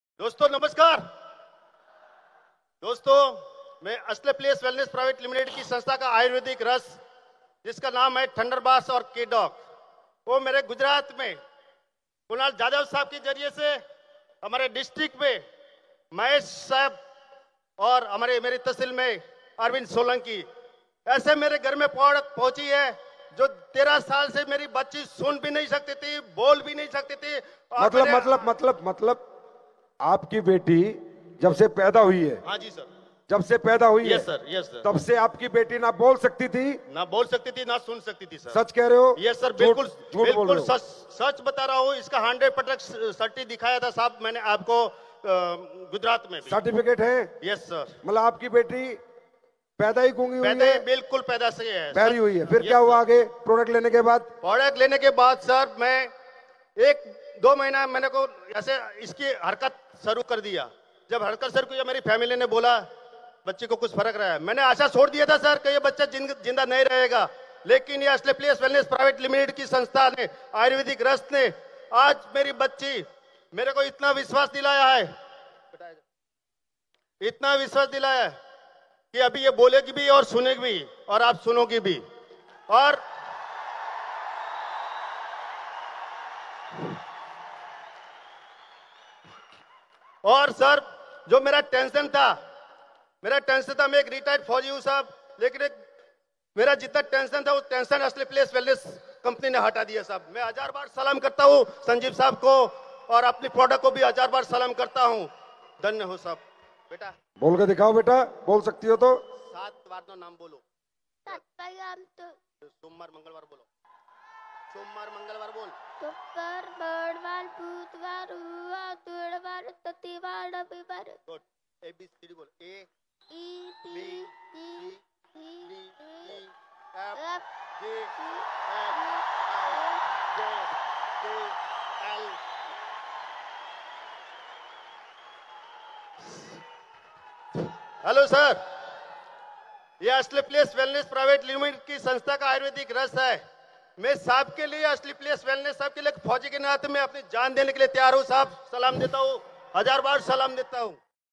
13 साल से बहरी, गूंगी लड़की आज बोलने,सुनने लगी। 9009009526
बताते हुए बड़ी खुशी हो रही हैं कि अस्क्लेपियस वैलनेस के प्रोडक्ट्स का उपयोग करके जो लड़की बचपन से बोल नही पा रही थी, सुन नही पा रही थी आज बोलती भी है, सुनती भी है। धन्य है संजीव कुमार सर आप जो इतने अच्छी जड़ी बूटी का उपयोग कर प्रोडक्ट बनाते है। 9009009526